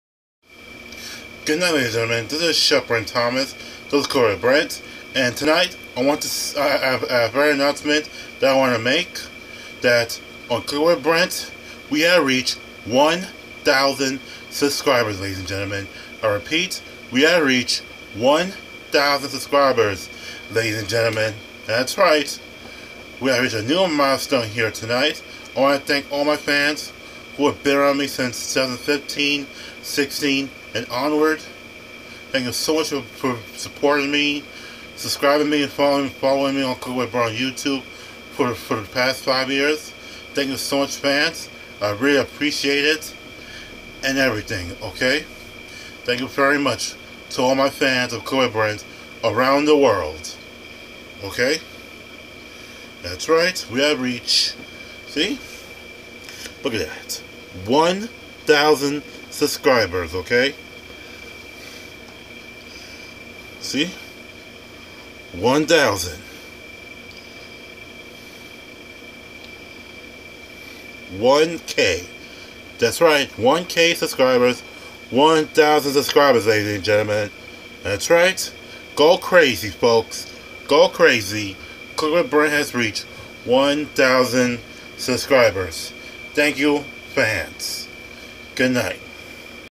Cooking With Brent's 1000th Subscriber Shout-Out
Today fans of Cooking With Brent we had reached our goal by getting 1,000 subscribers here tonight. We have set a new milestone and I want to thank all of my fans, friends, family and etc for helping me reach 1,000 subscribers on youtube. Chef Brent want to celebrate the moment with you on reaching our goal we had set five years ago. And today we had made from the bottom to the top of the world ladies and gentlemen, we had fulfill our promise here on Cooking With Brent and cheers to the next five years on Cooking With Brent and let's get more subscribers in the process as well. Real Talk 123. Thank fans for your continuing support and your cooperation as well. Thank fans of Cooking With Brent.